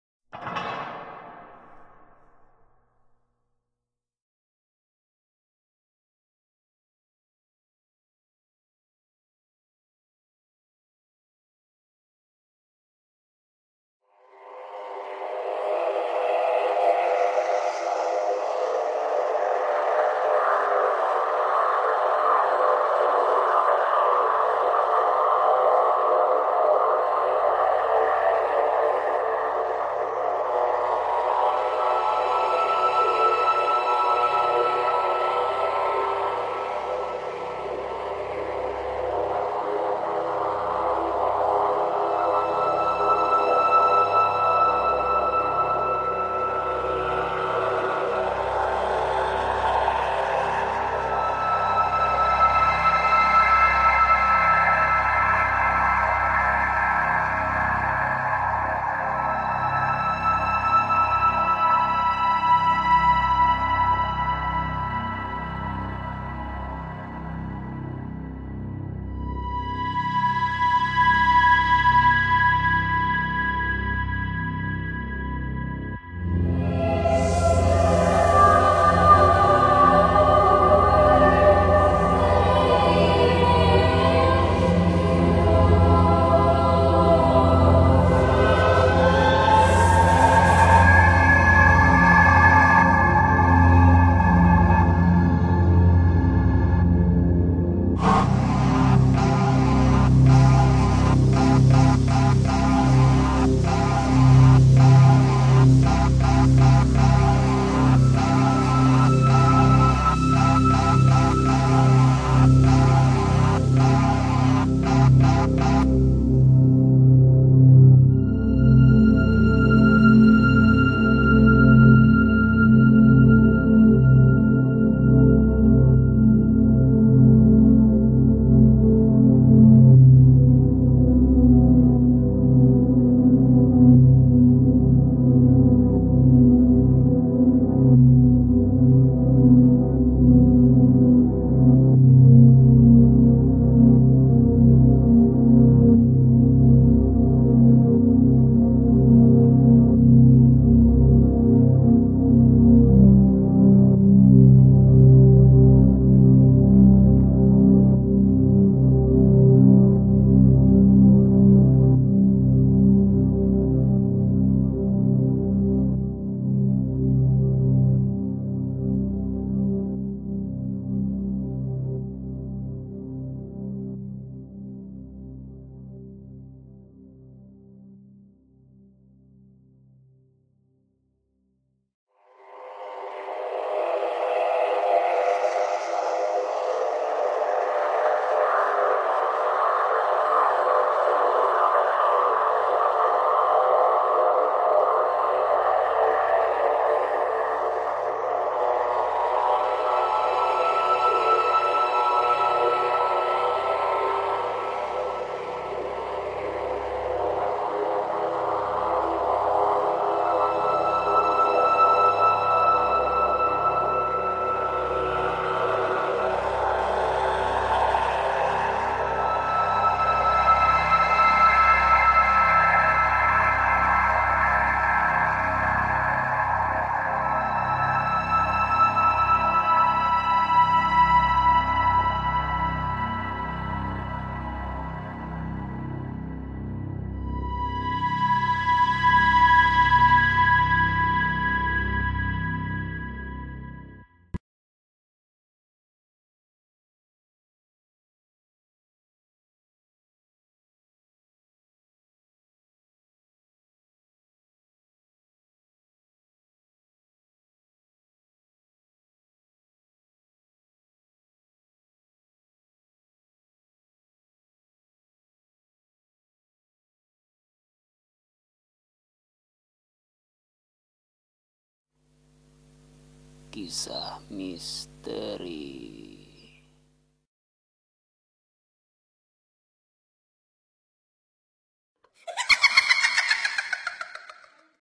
Cerita Seram - Bertapa di Goa dapat Ilmu Hitam - Kizmizz.17
Nekad bertapa di hutan terlarang... OMG...